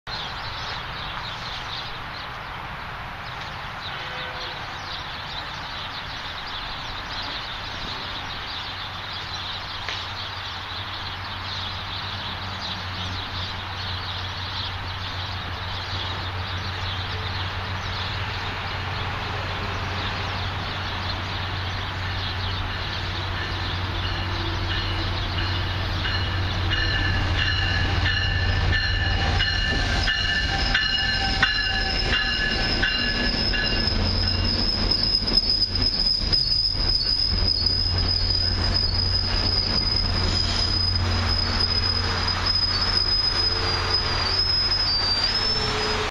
MBTA Commuter Rail Arriving At Auburndale With MP36PH Locomotive In Front
This is a Commuter Rail Train arriving at Auburndale in Newton, Ma. One of the New MP36PH locomotives are pulling this train.